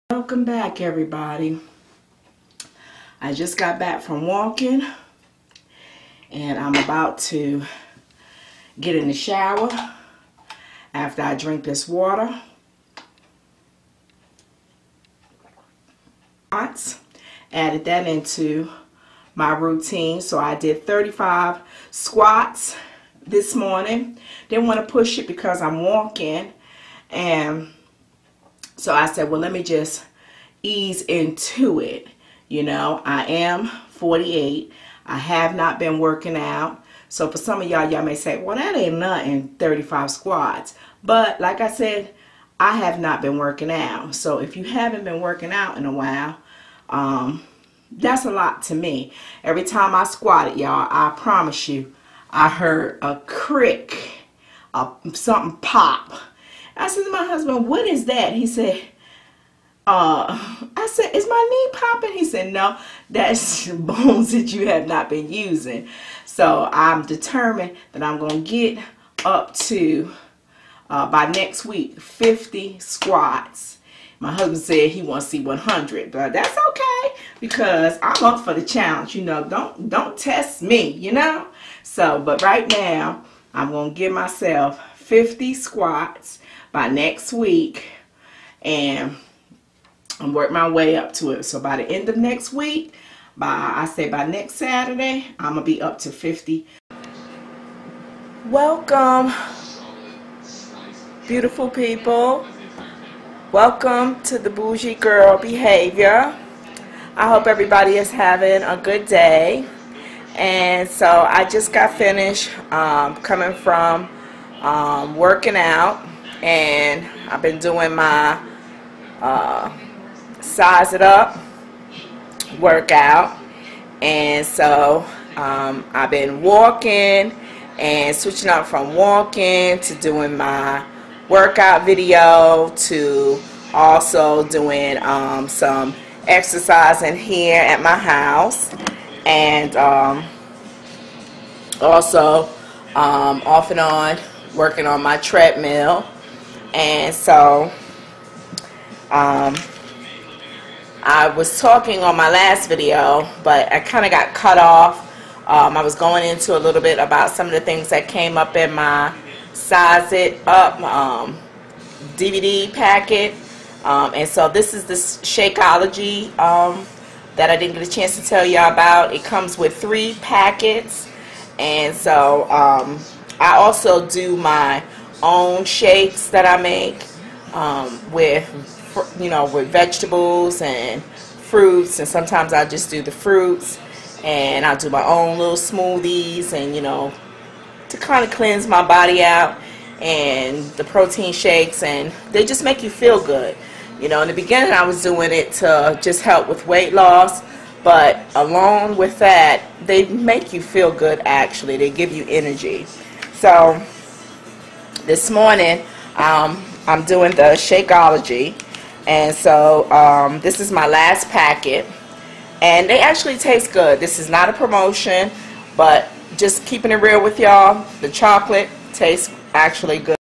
BABY STEPS TO A BETTER ME VLOG # 27
Working out which includes power walking, aerobics, tracking my daily steps, eating healthier, drinking more water, doing swats and protein shakes & juicing occasionally. I hope my videos will encourage & inspire whoever sees them.